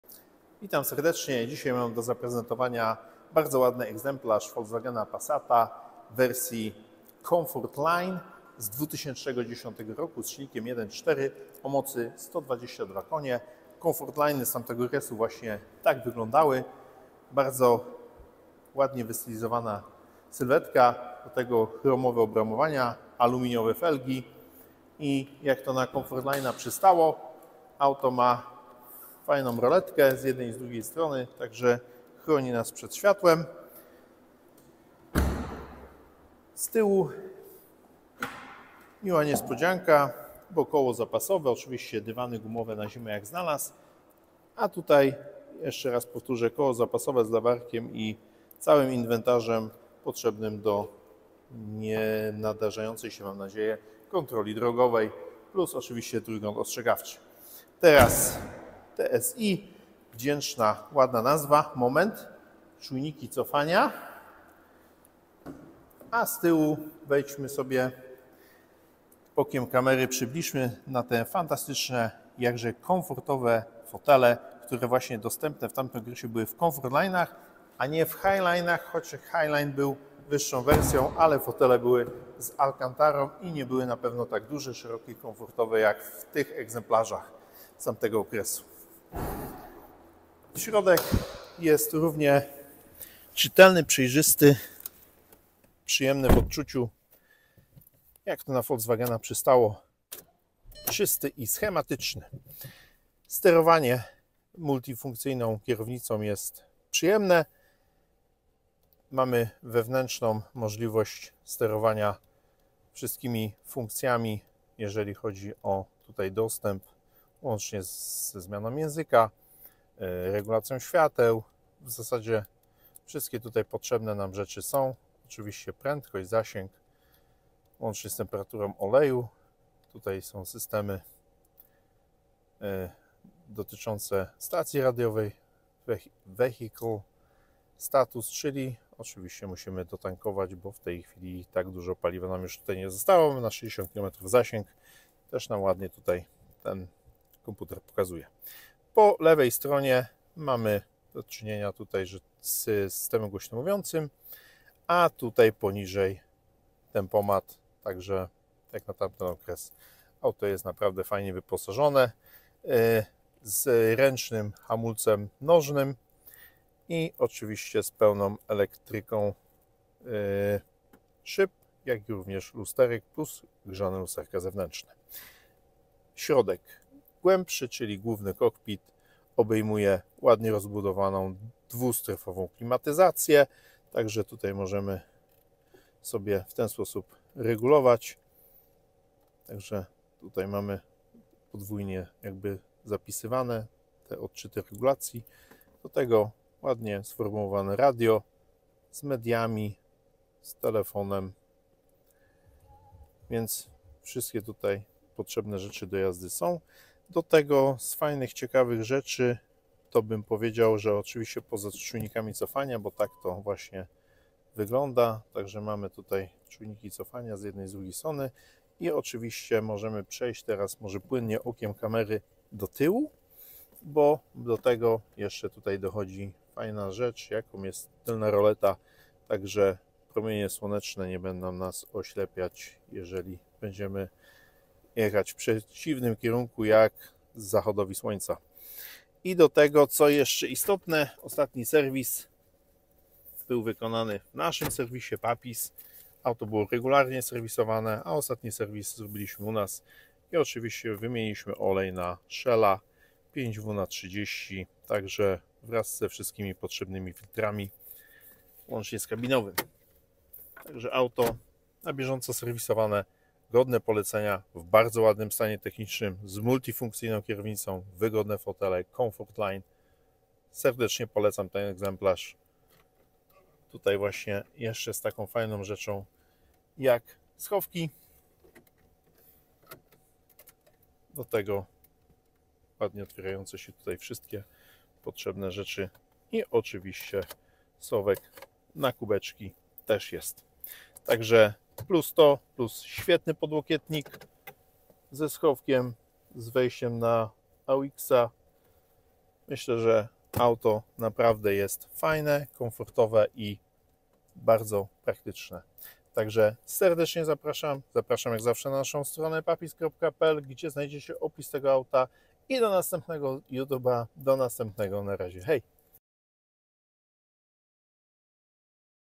VOLKSWAGEN PASSAT B6 SEDAN 1.4 TSI 122KM 2010’ Comfortline Manual SalonPL 22 900 PLN
VOLKSWAGEN PASSAT B6 SEDAN 1.4 TSI 122KM 90KW, skrzynia biegów manualna 6-stopniowa,Wersja wyposażenia: ComfortlineRok produkcji: 2010
Dane techniczne:Pojemność skokowa – 1390 cm³Typ silnika – benzynowyMoc silnika – 122 KM (90 kW) przy 5000 obr/minMaksymalny moment obrotowy – 210 Nm przy 4000 obr/minLiczba cylindrów – 4Układ cylindrów – rzędowyŚrednie spalanie (cykl mieszany) – 6,6 l/100km
Cena brutto = 22 900 zł